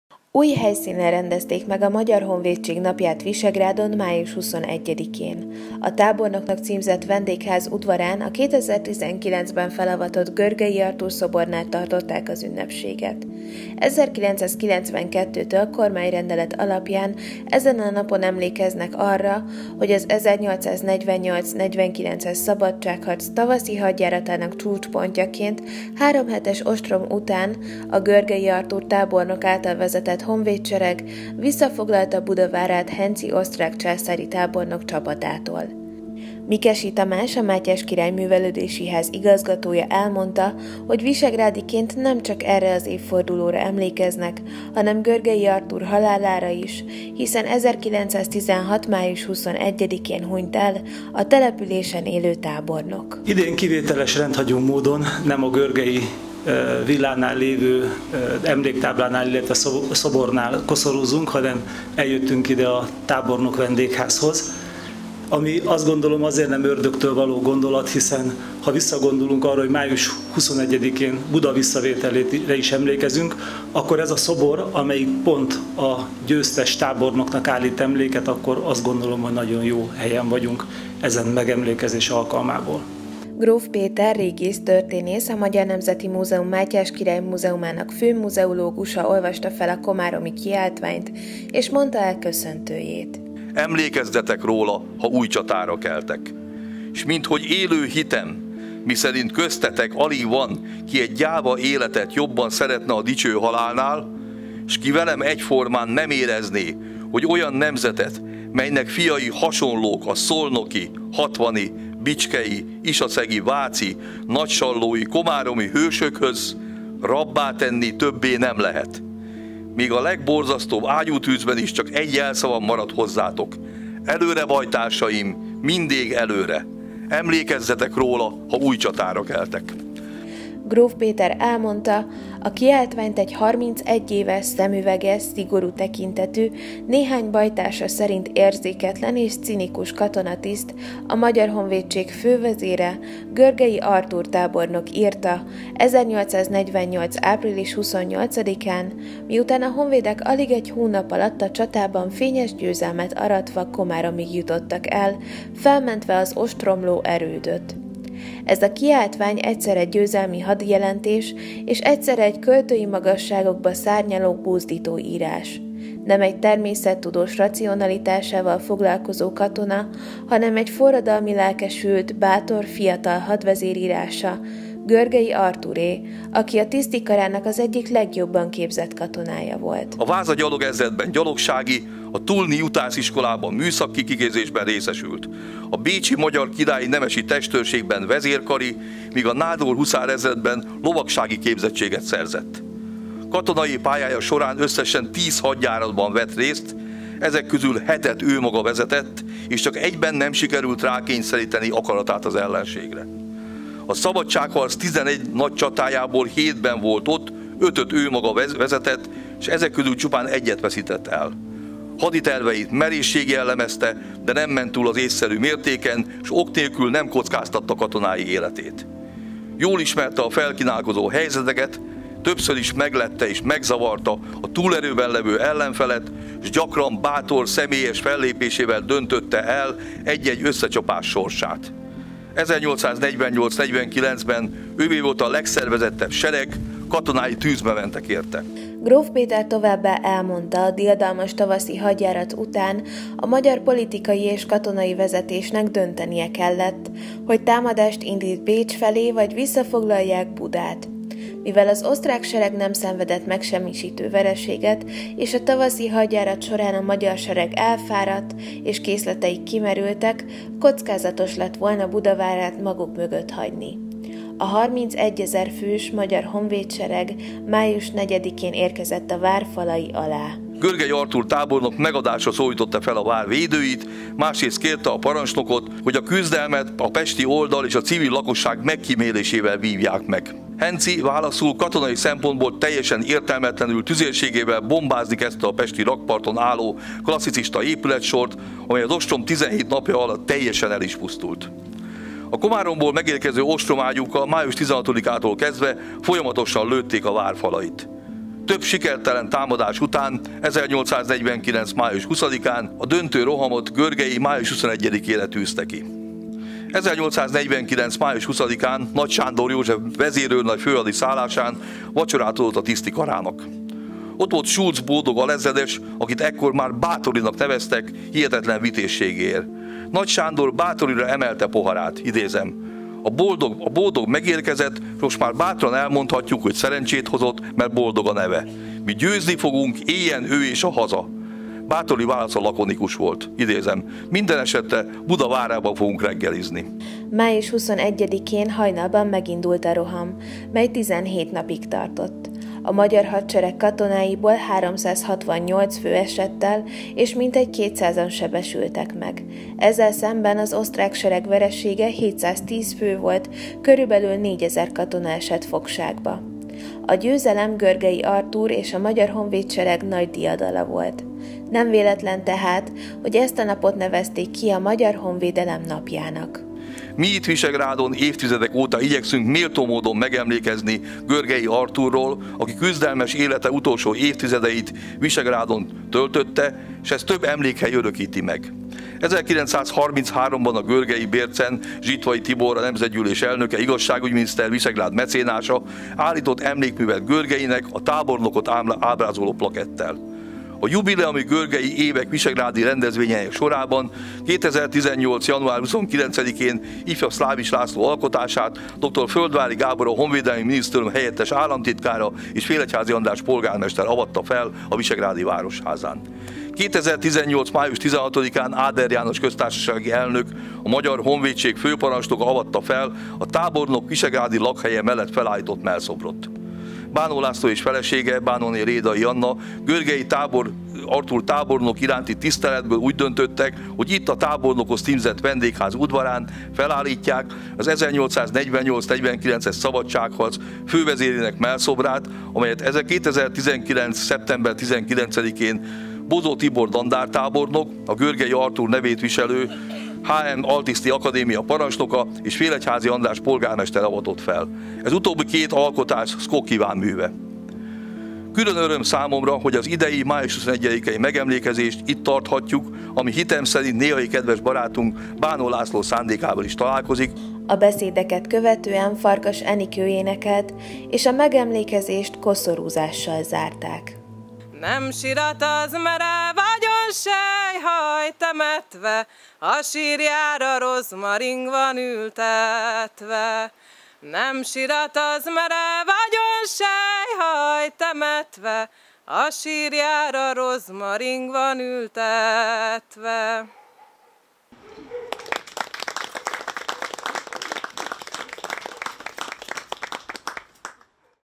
Megemlékezés Görgei Artúr honvédtábornok halálának és Buda visszavételének évfordulóján Visegrádon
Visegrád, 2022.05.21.